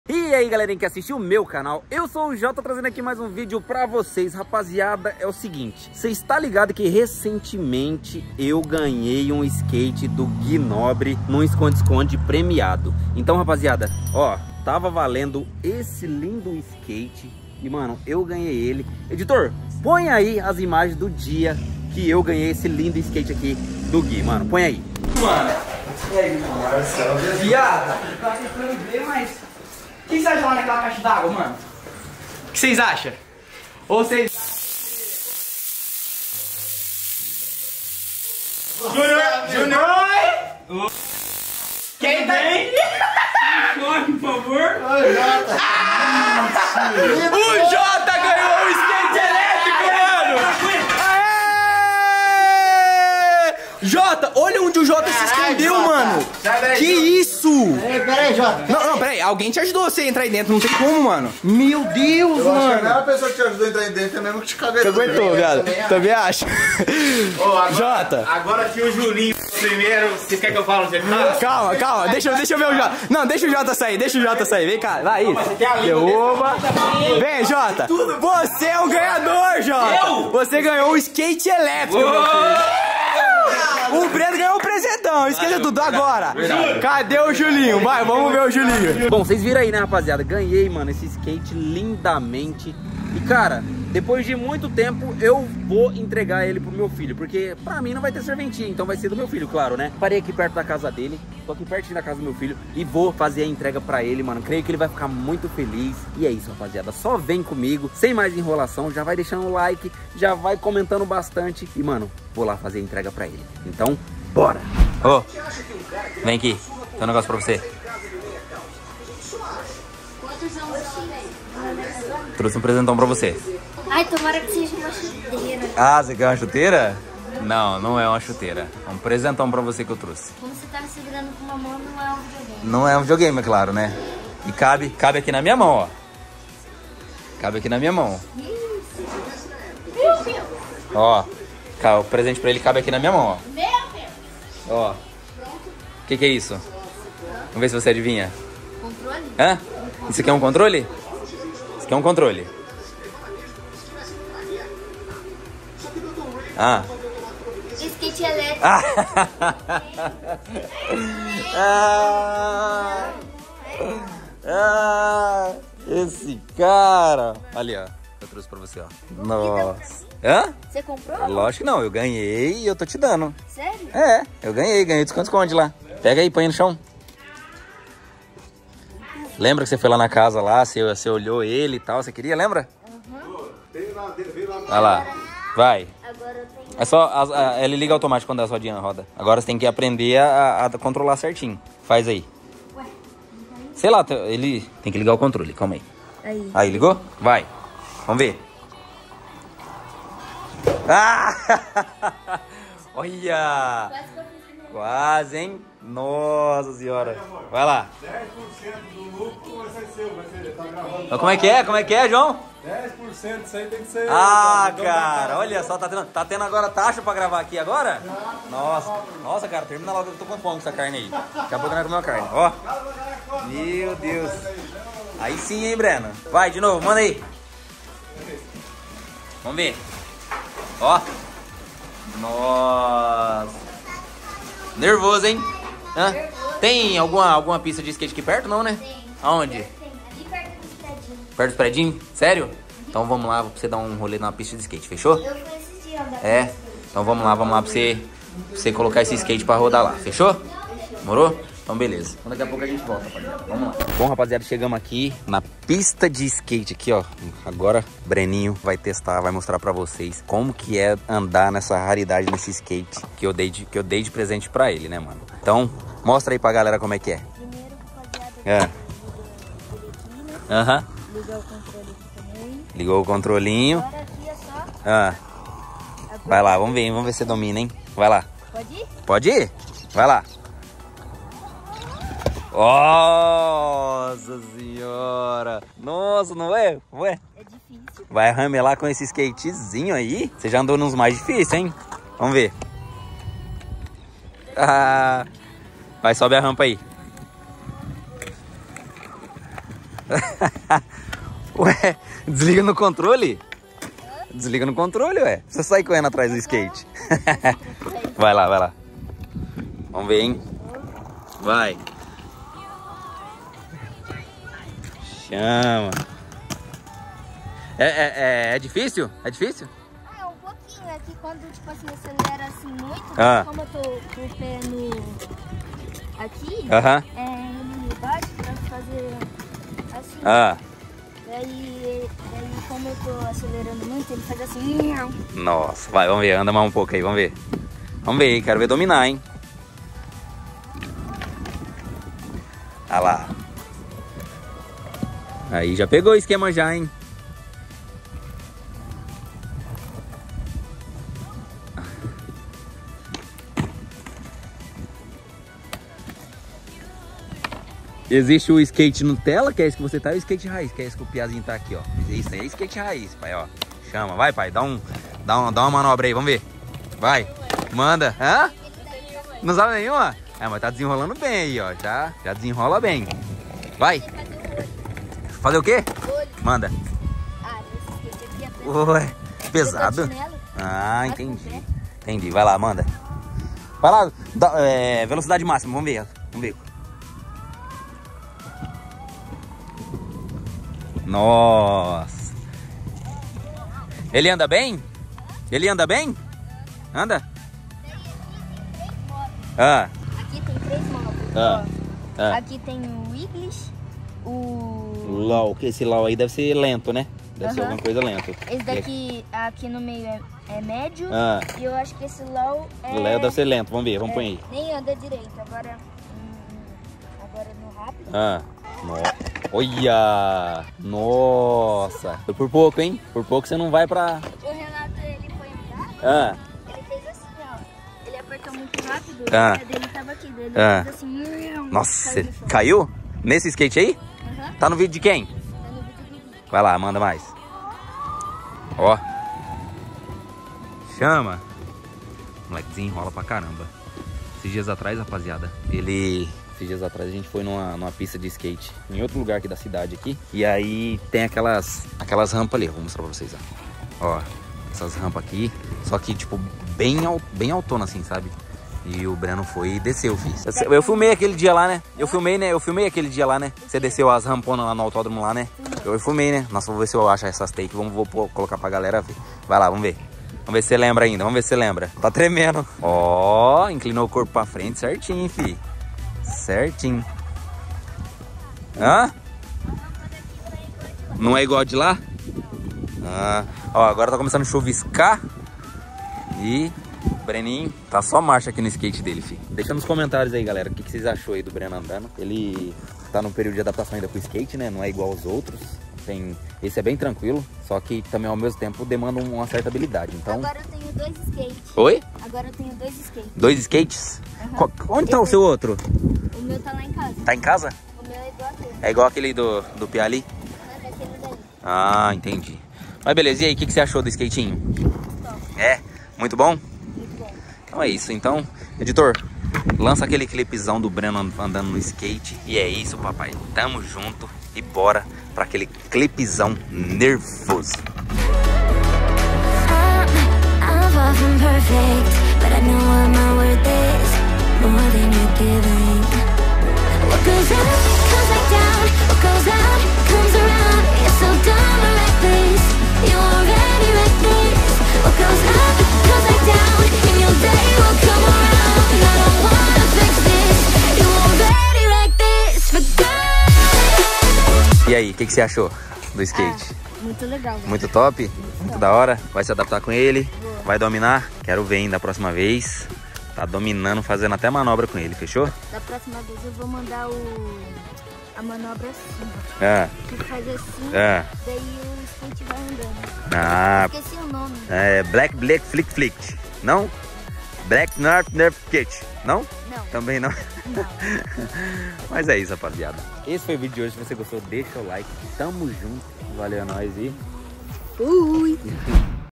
DEI DE PRESENTE PRO MEU FILHO O SKATE ELÉTRICO QUE GANHEI DO GUI NOBRE…
E ai galerinha que me acompanha,
Toma mais um videozão pra vocês!!